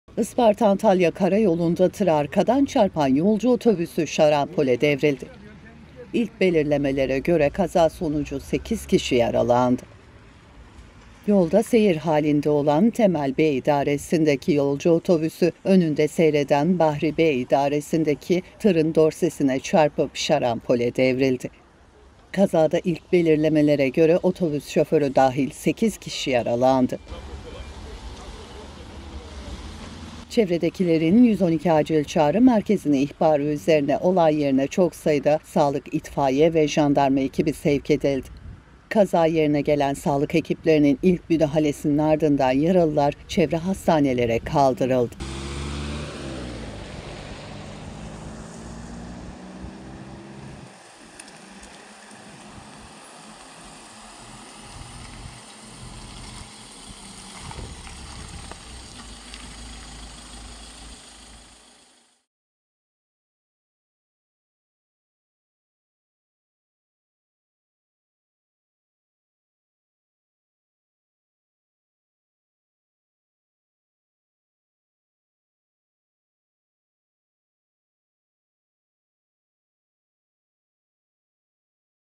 Isparta Antalya Yolunda Yolcu Otobüsü Şarampole Devrildi : 8 Yaralı
Isparta-Antalya kara yolunda tıra arkadan çarpan yolcu otobüsü şarampole devrildi. İlk belirlemelere göre kaza sonucu 8 kişi yaralandı.

Kaza, saat 12.50 sıralarında Burdur'un Bucak ilçesi Çobanpınar köyü Isparta-Antalya kara yolunda meydana geldi. Edinilen bilgilere göre, yolda seyir halinde olan Temel B. (45) idaresindeki 07 BIN 659 plakalı yolcu otobüsü, önünde seyreden Bahri B. (27) idaresindeki 32 LT 363 plakalı tırın dorsesine çarpıp şarampole devrildi. Kazada ilk belirlemelere göre otobüs şoförü dahil 8 kişi yaralandı.
Çevredekilerin 112 Acil Çağrı Merkezine ihbarı üzerine olay yerine çok sayıda sağlık, itfaiye ve jandarma ekibi sevk edildi. Olay yerine gelen sağlık ekiplerinin ilk müdahalesinin ardından yaralılar çevre hastanelere kaldırıldı.